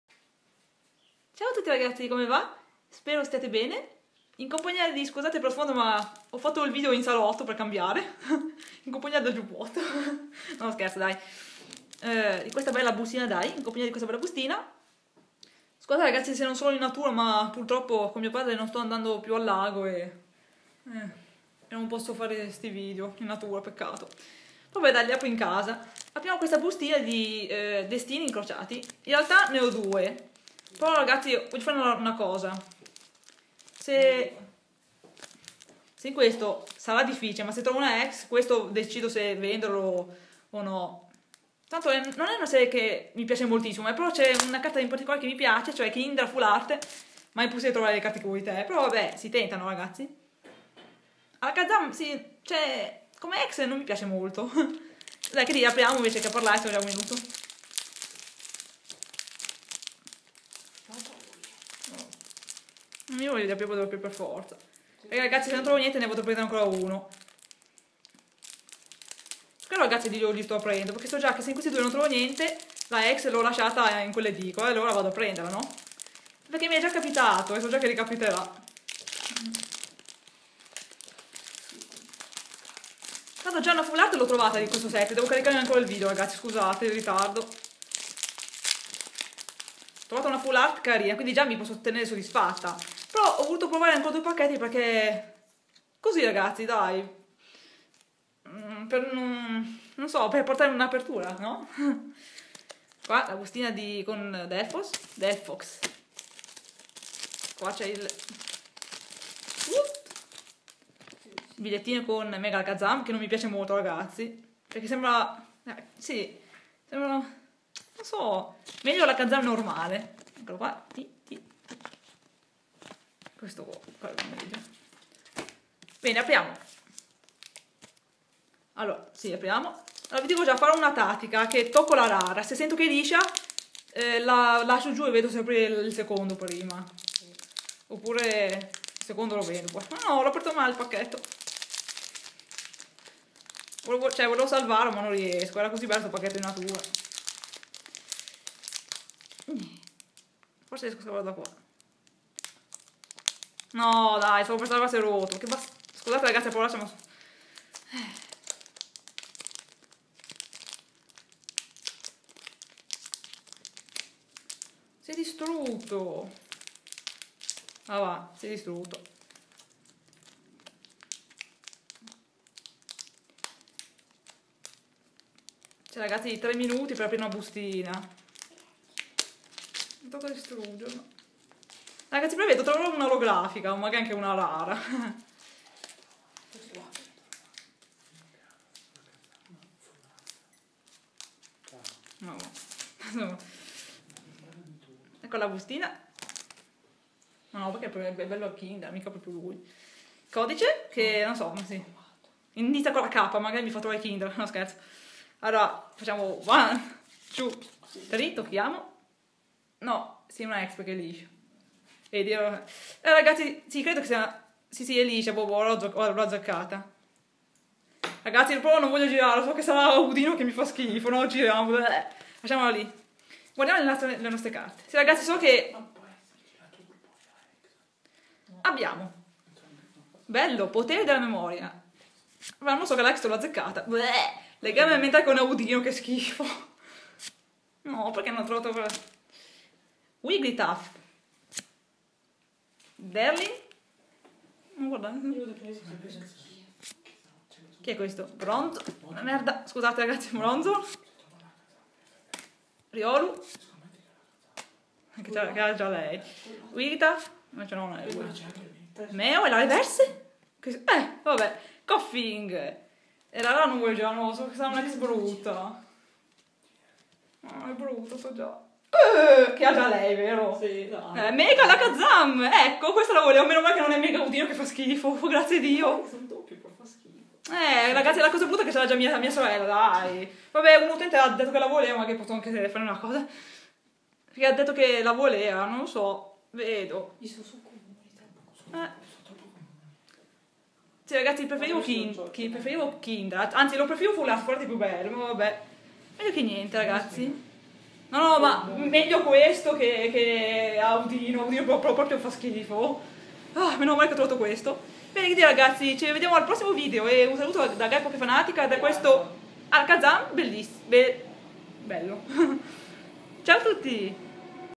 Apertura 1 bustina Pokemon xy Destini Incrociati (Mega)
Ciao a tutti spero stiate bene :D che dire.. apertura stupenda..dal vivo è carino davvero :D sono contenta di averlo trovato *-* (SE PARLO VELOCE è L'EMOZIONE XD) buona visione a tutti!!